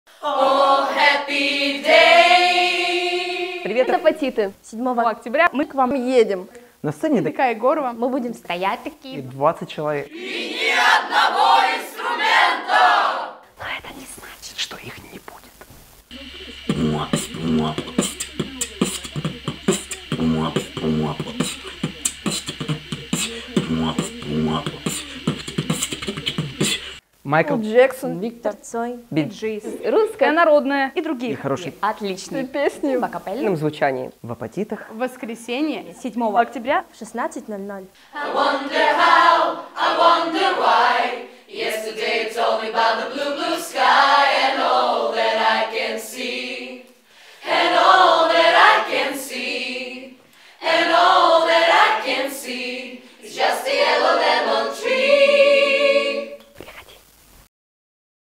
The Хор едет в Апатиты
7 октября в 16:00 на сцене Апатитского ДК им. Егорова.